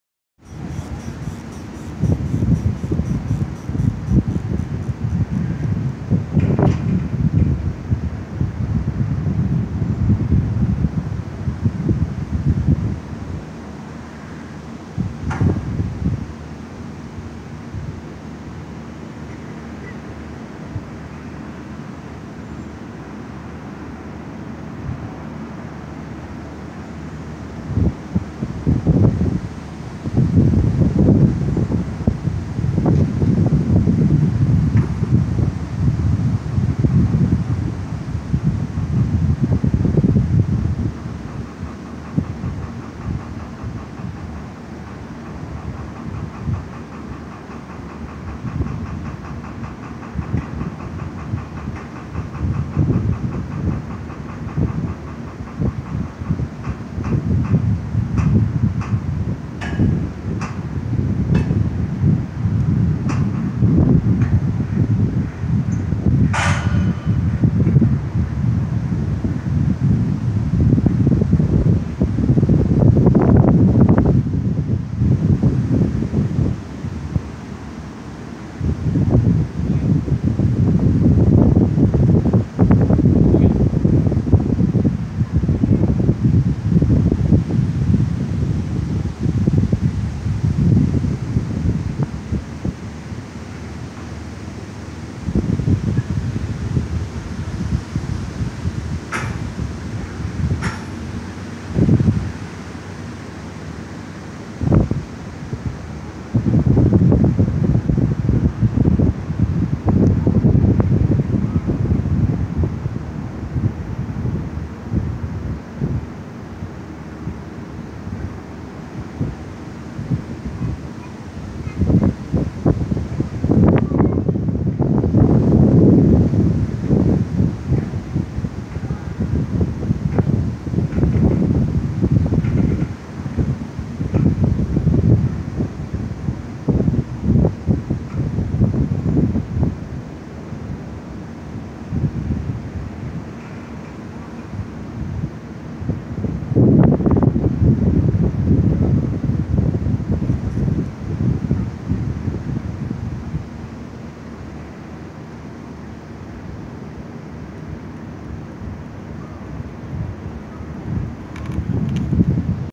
Voyageur Park Drawbridge De Pere
This recreational park Drawbridge is going up for a yacht to pass through.